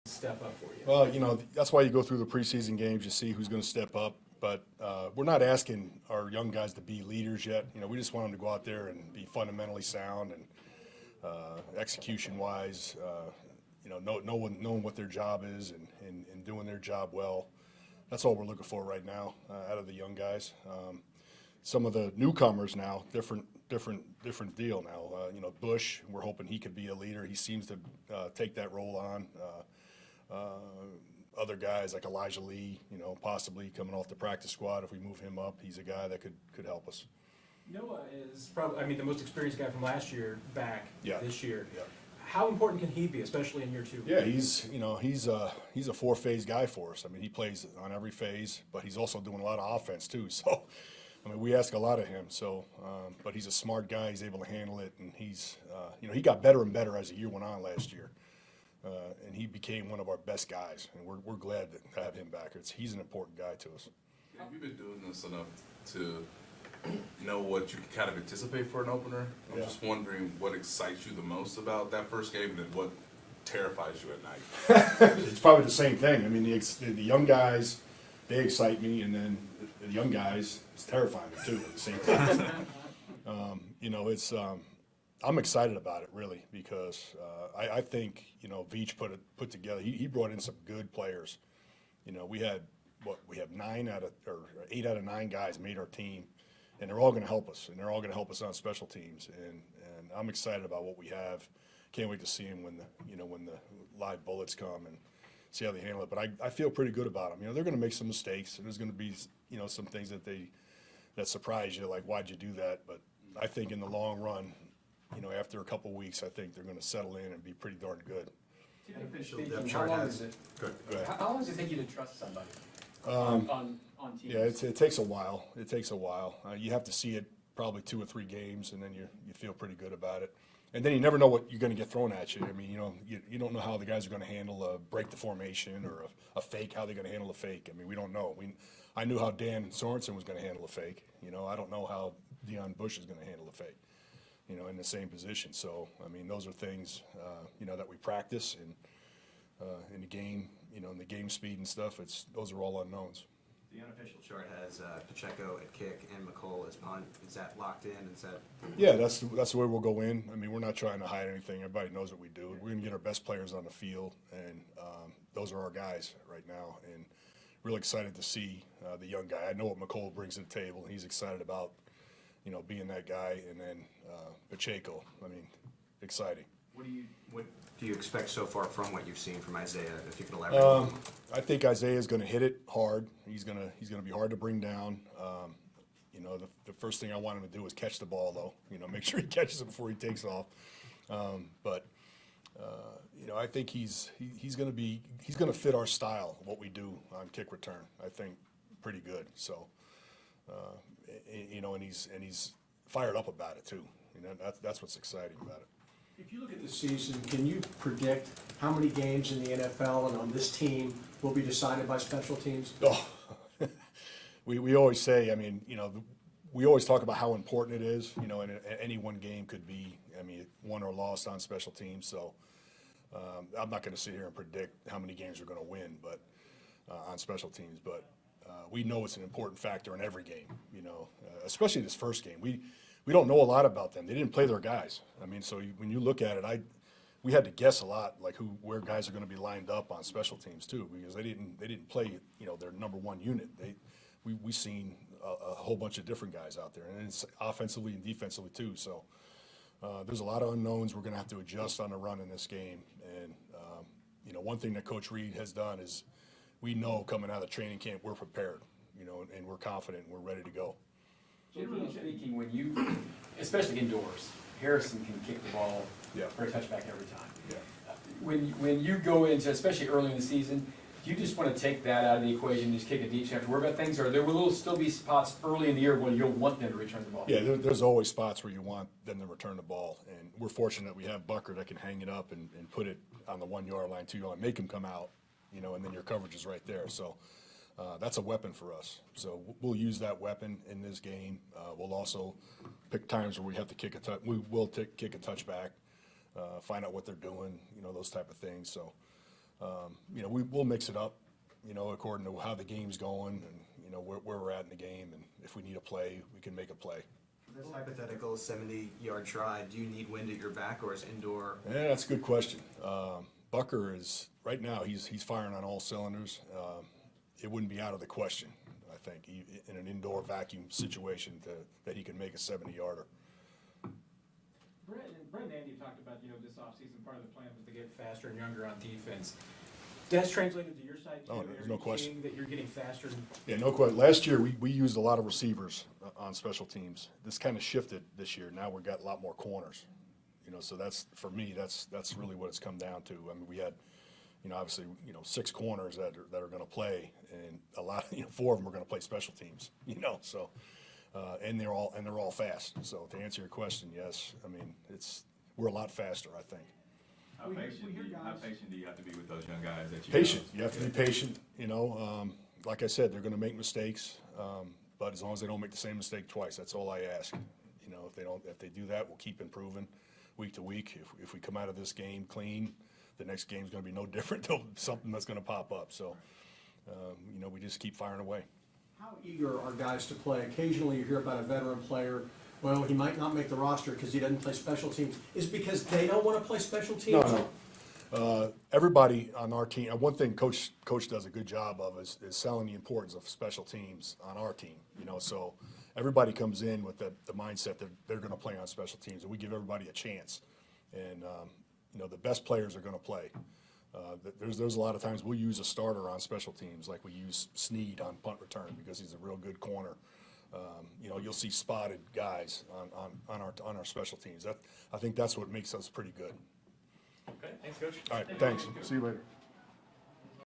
Toub: “If we can get our best players on the field then those are our guys.” | Press Conference 9/8
Assistant Head Coach and Special Teams Coordinator Dave Toub speaks with the media ahead of the Chiefs Week 1 matchup against the Arizona Cardinals